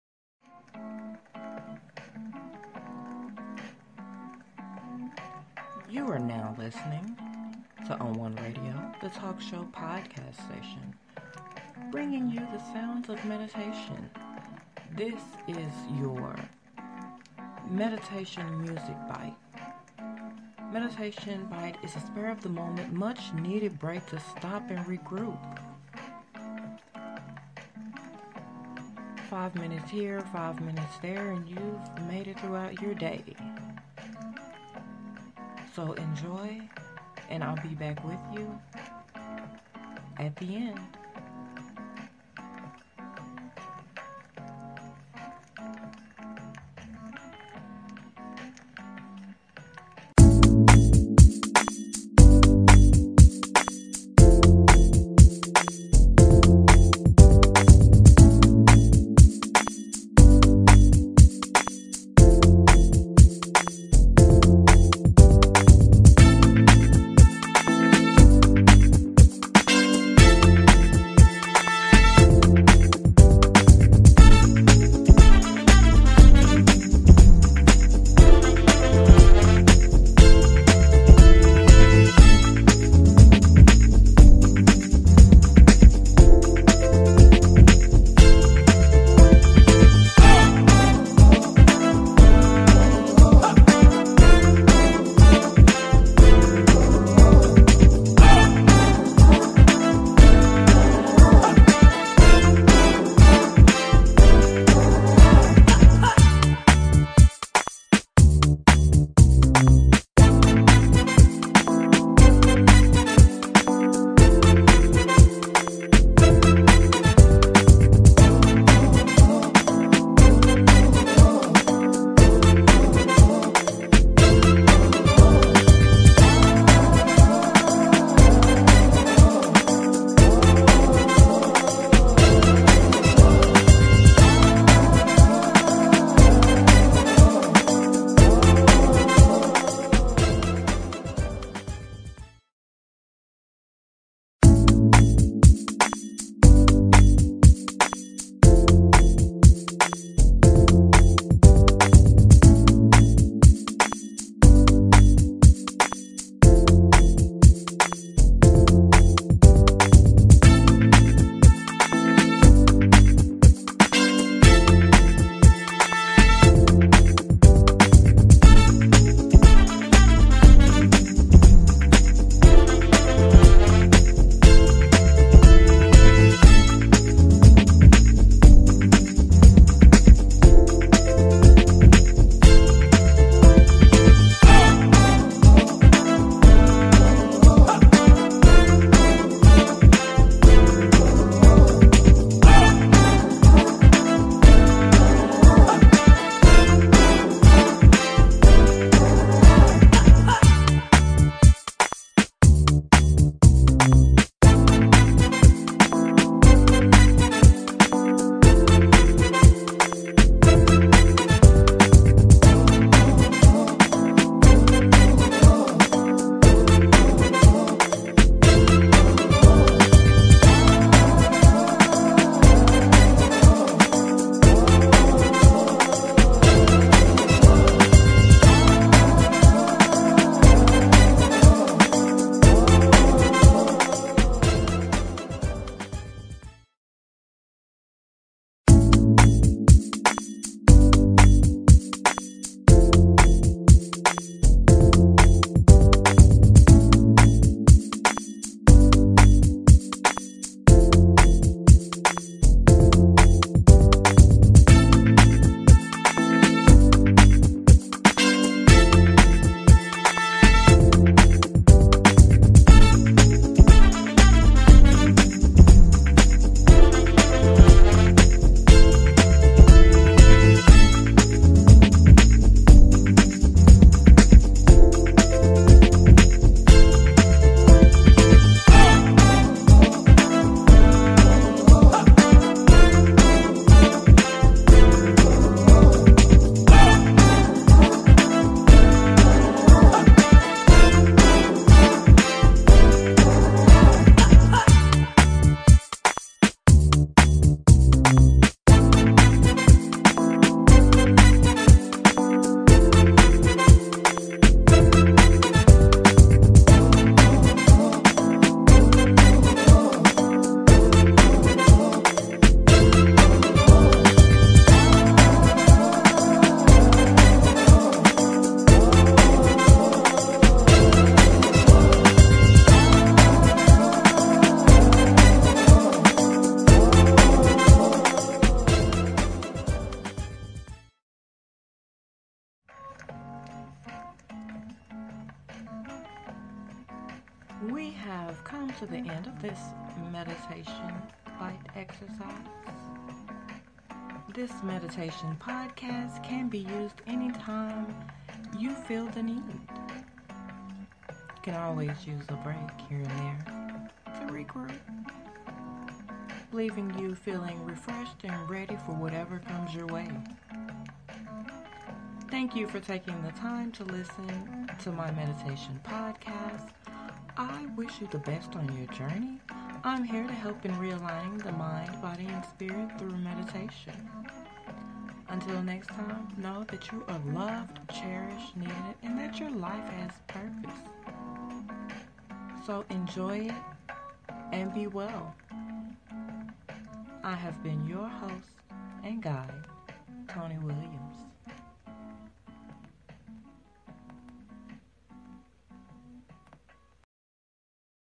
Uplifting Meditation Music Bite 6 mins
Uplifting and Insperational Meditation Music Bite for six minutes of much needed relaxation quickly. Meditation Bite is a spare of the moment needed brak to stop and regroup. It is great for all types of meditation practice, breathe, relax ,enjoy and be well.
Sound Meditation